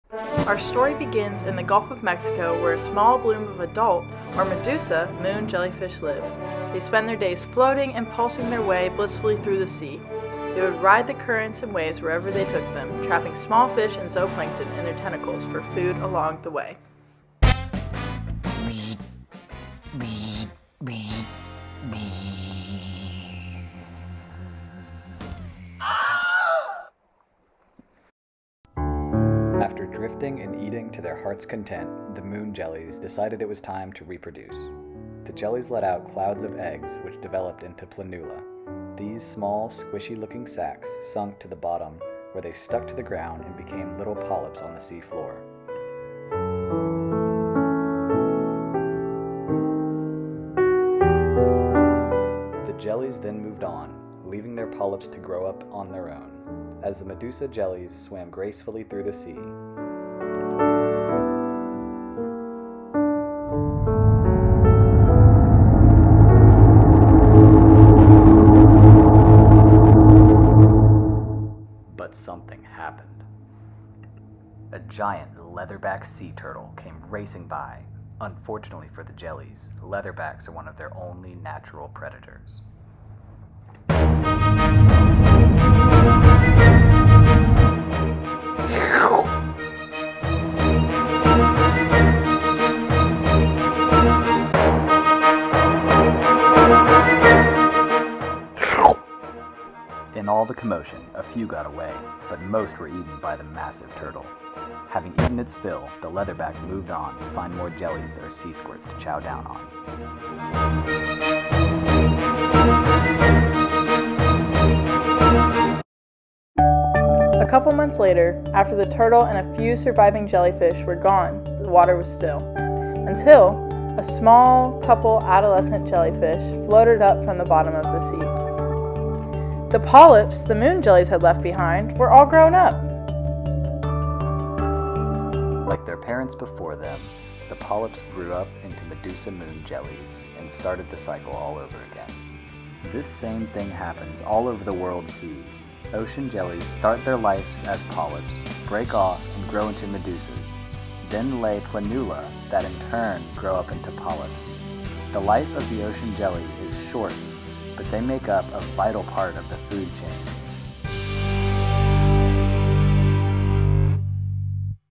Ocean Jellies
Fall 2021 Wonder Challenge: Ocean Jellies

This video was created for the Wonder Challenge for the Fall 2021 Environmental Science, Politics, and Policy course taught by Dr. Kwame Owusu-Daaku at the University of West Florida. We partnered with Elizabeth Eubanks and 6th grade students from Dixon School of Arts and Sciences. We created a video answering the question posed by the 6th graders “I wonder…what are sea/ocean jellies?”

Our video explains the life cycle of sea/ocean jellies through the playful yet innovative medium of a puppet show.

Team members: Megan Gordon, Noah White, Jade Stone-Shaver, Anna List, and Jessica Marquis